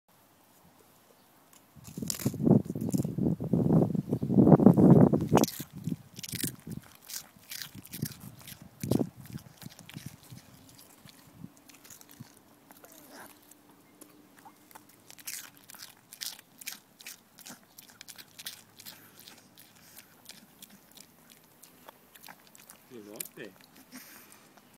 Yum yum I love cheezels - Cassie the Brussels griffon shows
Yum yum I love cheezels - Cassie the Brussels griffon shows how to get the best crunch